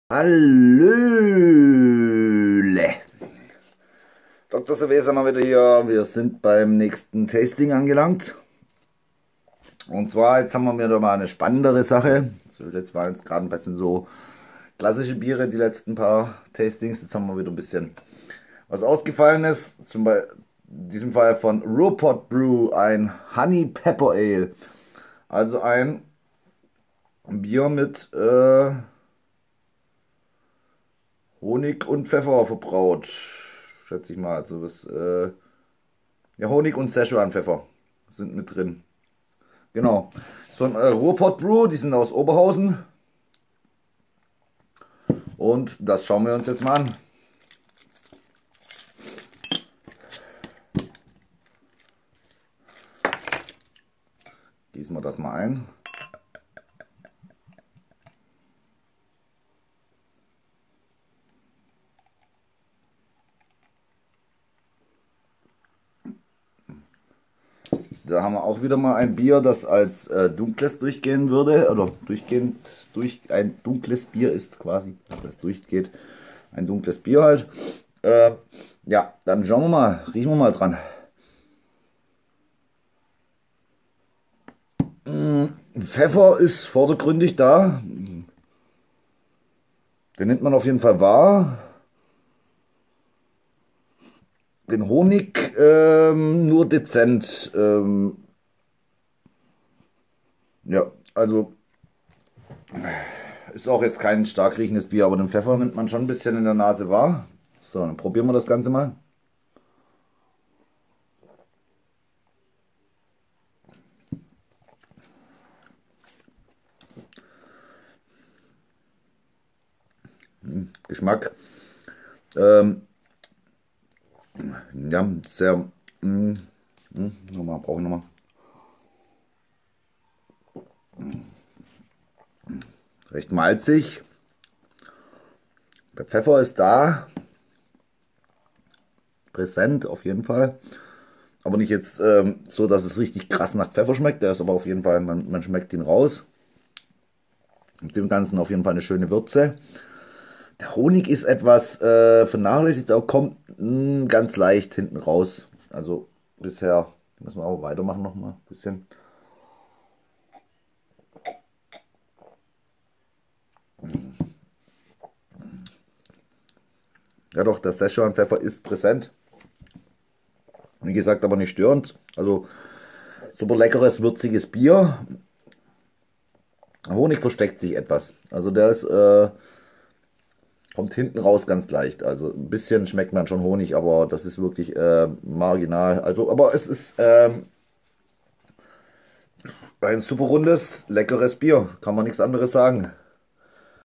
Ruhrpott Brew Honey Pepper Ale
Schönes Ale mit Honig und Szechuan Pfeffer verbraut mit 6,9% Alkohol